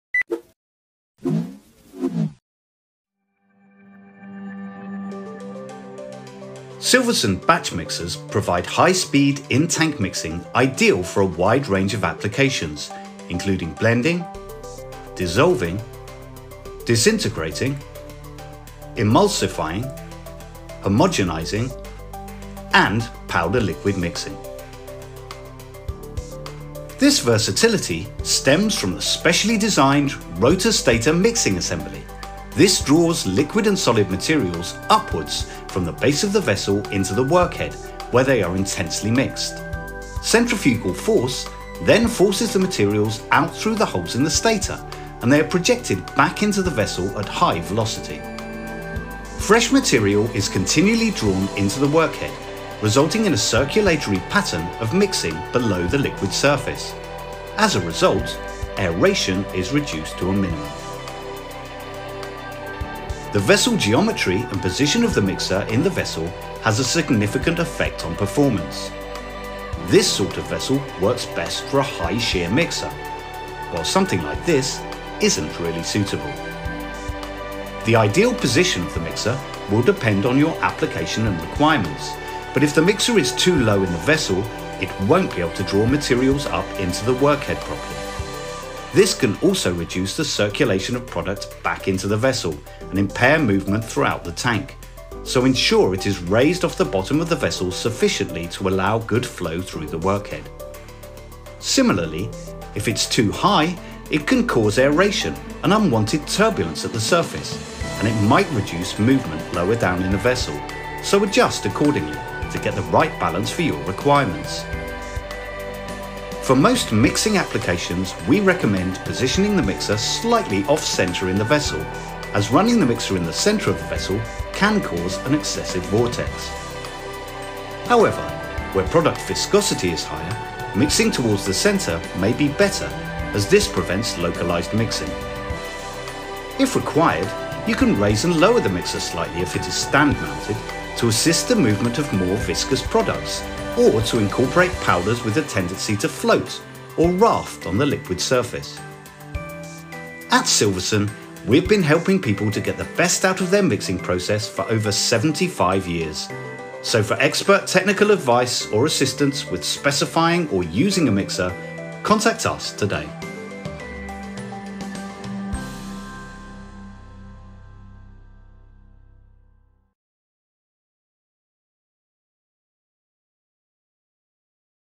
How to Use an In-Tank Batch Mixer – Industrial Mixing Guide
Achieve Optimal Mixing Results with Silverson Batch Mixers

Silverson Batch Mixers deliver high-speed in-tank mixing that’s ideal for a wide range of applications—blending, dissolving, disintegrating, emulsifying, homogenizing, and powder/liquid mixing. This performance is made possible by Silverson’s precision-engineered rotor/stator workhead, which ensures rapid and consistent mixing with minimal aeration.

But how do you make sure you're getting the best performance from your mixer?

In this video, we break down how vessel geometry and mixer positioning directly impact mixing efficiency—helping you get the most from your process.

In this video, you’ll learn how to:
✅ Reduce aeration and improve product consistency
✅ Maximize circulation and flow through the workhead
✅ Optimize mixer height for better mixing performance
✅ Prevent vortexing and turbulence
✅ Adjust mixer position for different viscosities

▶️ Chapters:
0:00 – What are Batch Mixers Used For?
0:27 – How the Rotor/Stator Workhead Works
0:49 – Mixing Circulation and Reduced Aeration
1:03 – Importance of Vessel Geometry
1:18 – Ideal Mixer Height in the Vessel
1:56 – Off-Center vs. Center Positioning
2:07 – Adjustments for High Viscosity Products
2:30 – About Silverson

Whether you're mixing low-viscosity liquids or thicker products, proper setup is key.